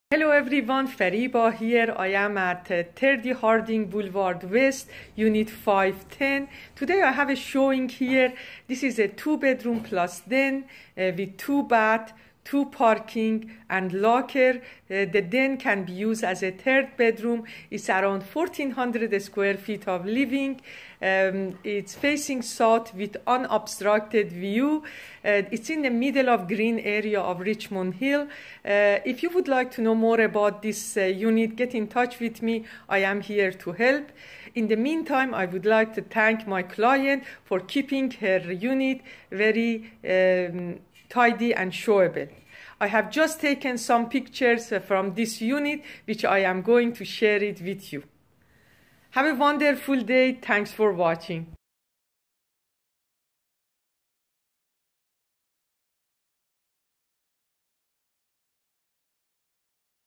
30 Harding Blvd. W #510 on April 3/2019
Hello Everyone,

Fariba here. I am at 30 Harding Blvd. W # 510 and have a showing today. A 2 Bed plus Den with 2 bath,  2 parking and locker. Facing south. Any  information you need, I am here to help.
Thank to my client who has kept her unit tidy and show-able.